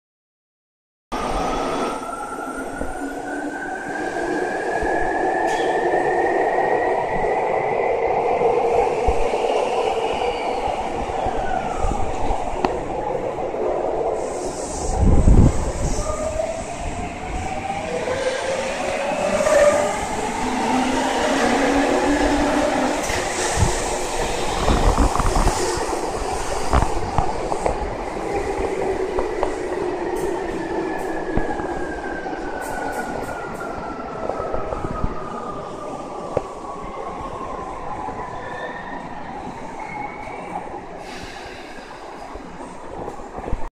｢急行運用代走｣東急大井町線9000系9103F 当駅止まり回送 溝の口発車
2016/5/1 撮影
この日東急大井町線9000系9103Fが大井町線急行124運用に代走しました。